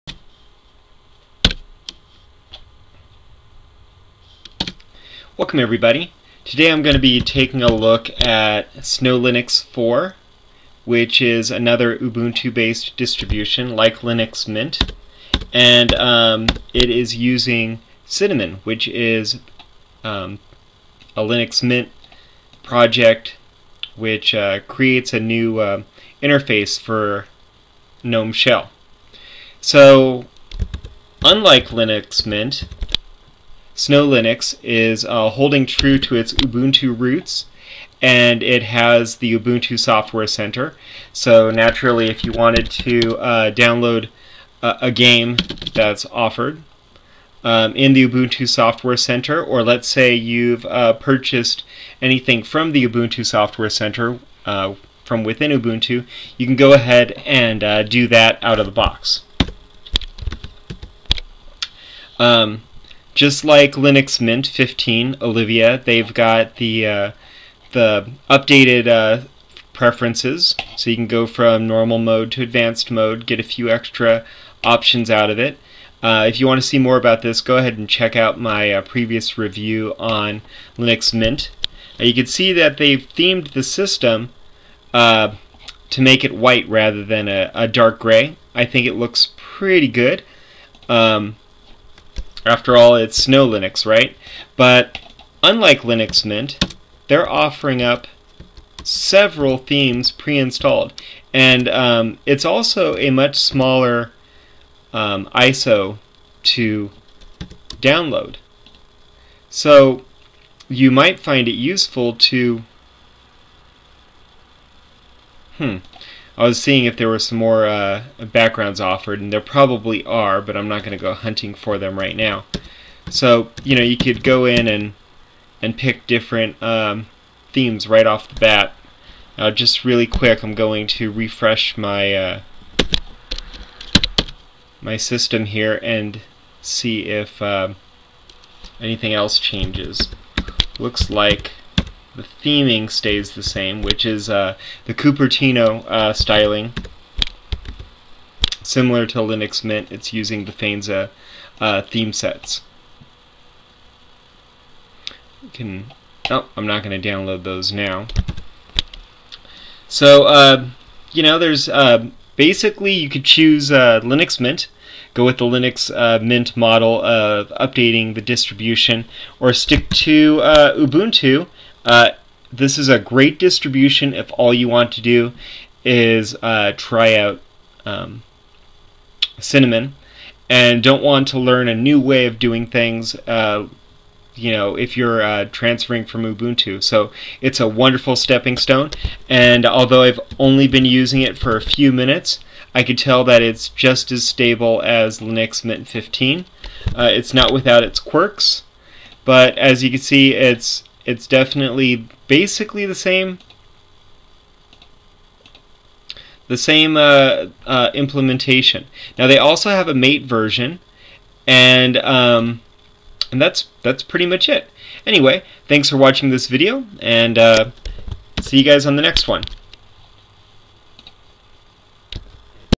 Snow Linux 4 "Frosty" Distribution Preview
A really quick tour around Snow Linux 4 "Frosty"
Cinnamon and MATE desktops are built upon Ubuntu 13.04 Raring Ringtail so you don't have to give up the Ubutu experience when all you dislike is Unity.